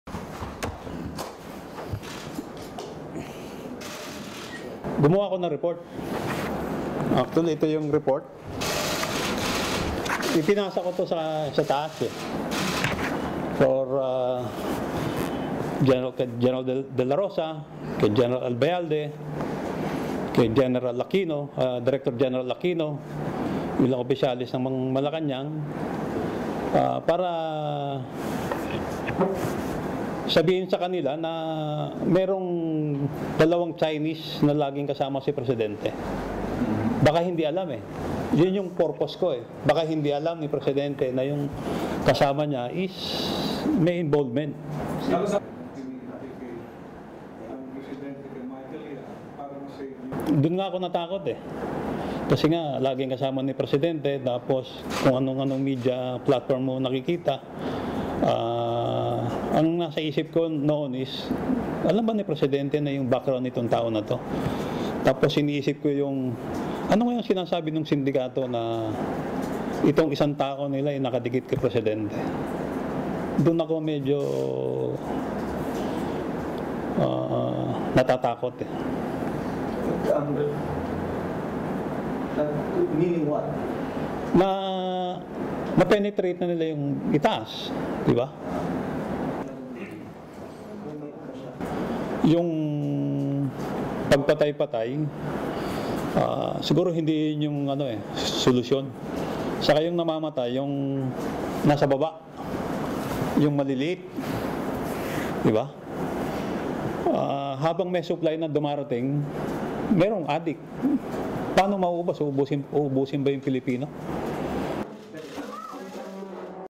Ex-official sent Duterte report on drug-linked men
(25 Mar 2019) A former senior Philippine police official says President Rodrigo Duterte has been photographed with two Chinese men involved in illegal drugs and sent a report to warn him.
Dismissed Senior Superintendent Eduardo Acierto said on Sunday that he's unaware what government action has been taken after he submitted his report about the Chinese men to top police officials starting in December 2017.
Instead, Acierto said he's now facing illegal drugs complaints and has been the target of death threats that forced him to go into hiding recently.
"I made a report o inform them that there are two Chinese who are always accompanying the president," said Acierto, who was a veteran anti-narcotics official before he was dismissed last year by an anti-graft agency.
"In my mind at the time was 'does the president know about the background of this person?'"
Acierto's confidential report about the alleged drugs links of the two Chinese contained details including the involvement of one of them in the establishment of a secret methamphetamine drugs manufacturing lab raided by authorities in 2004.
Philippine Drug Enforcement Agency Director General Aaron Aquino told The Associated Press that he received Acierto's report and sent it to Duterte's office, adding that both his office and that of the president took steps to validate the allegations against the two Chinese.
An initial check showed that at least one of the Chinese was not facing any illegal drugs case in the country, he said.
More than 5,000 drug suspects have been killed during drug raids under Duterte's crackdown.
Duterte has denied condoning extrajudicial killings but has openly threatened drug suspects with death.